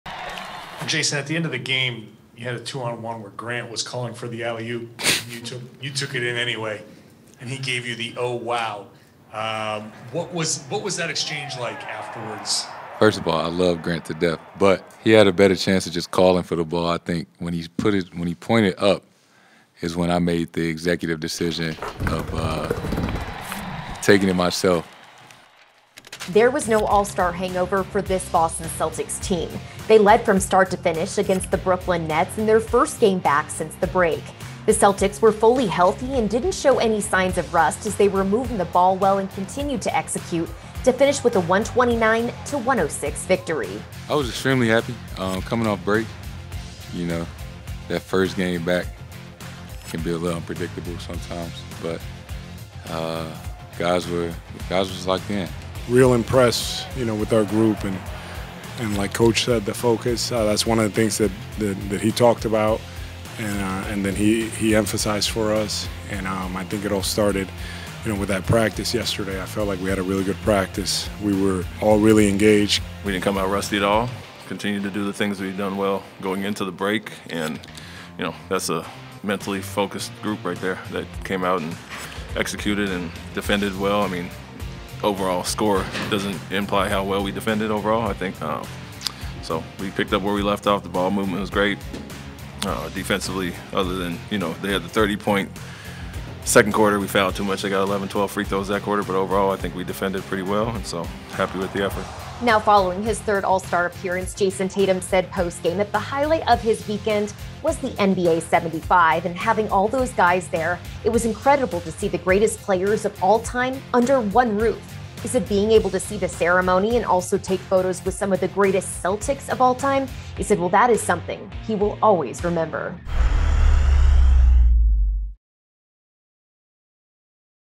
2/24 Putnam Postgame Report: ‘Happy with the Effort’
The Celtics avoided an All-Star hangover and picked up right where they left off by putting together a 129-106 victory over the Nets.